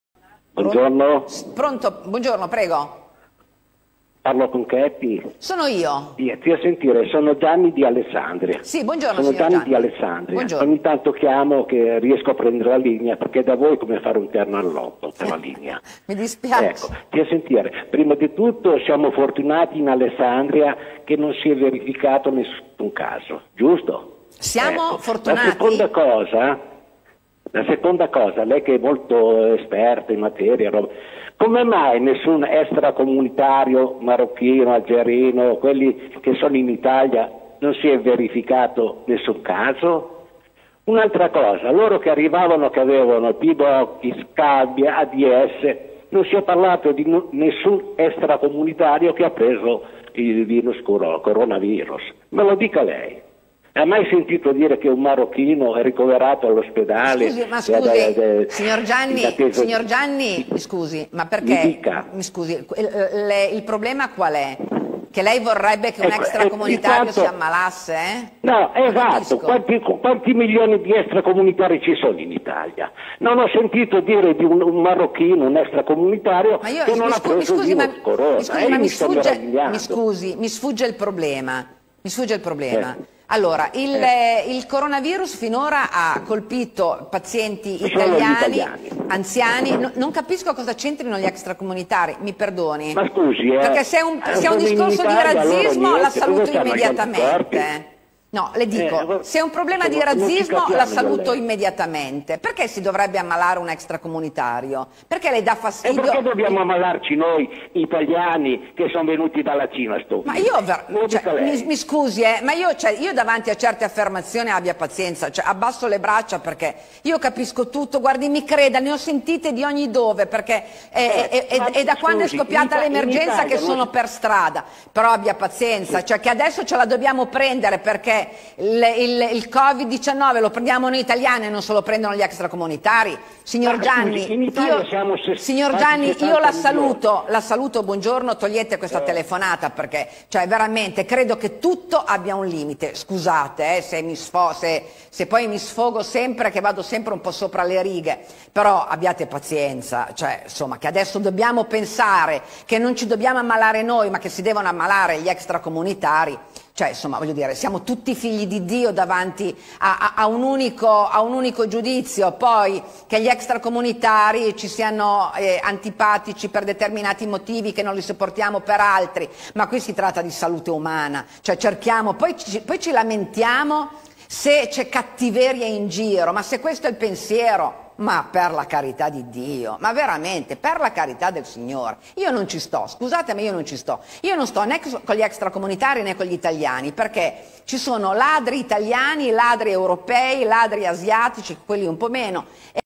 CORONAVIRUS vs RAZZISMO verso gli extra comunitari in Italia
Recentemente sul sito abbiamo pubblicato informazioni sulle 3 cose più importanti che bisogna conoscere per evitare la propagazione dell'epidemia di coronavirus o Covid-19. Ma, nel frattempo abbiamo visto che alcune persone non sono contente perché fino adesso non ci sono gli immigrati o stranieri con questo virus. I razzisti non lo capiscono per niente perché non sanno che i più colpiti sono gli anziani e tanti extra comunitari sono giovani. ASCOLTATE il signorGIANNI e la risposta saggia della giornalista a proposito del coronavirus. Trattasi di razzismo?